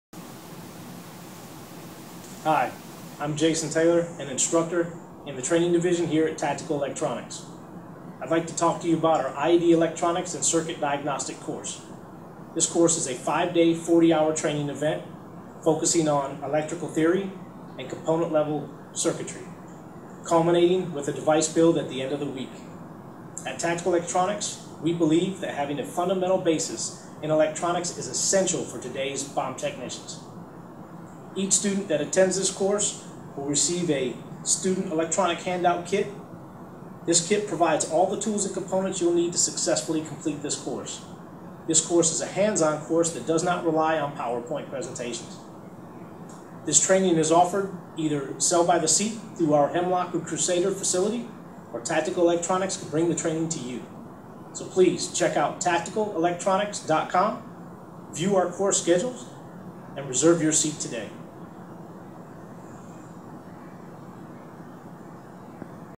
IED Electronics & Circuit Diagnostics Training Course | Tactical Electronics
See instructor, Jason Taylor describe Tactical Electronics IED Electronics & Circuit Diagnostics Course. to register for the course.